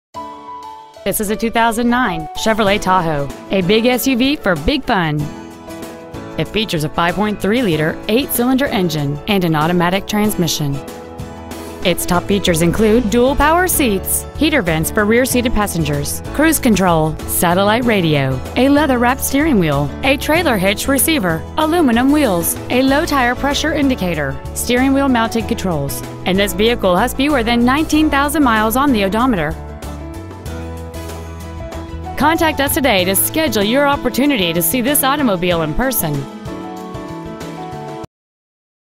2009 Chevrolet Tahoe Chattanooga TN
Year: 2009
 Make: Chevrolet
 Model: Tahoe
 Engine: 5.3L V8
 Trans.: Automatic
 Exterior: Maroon
 Miles: 18,082
 Interior: Black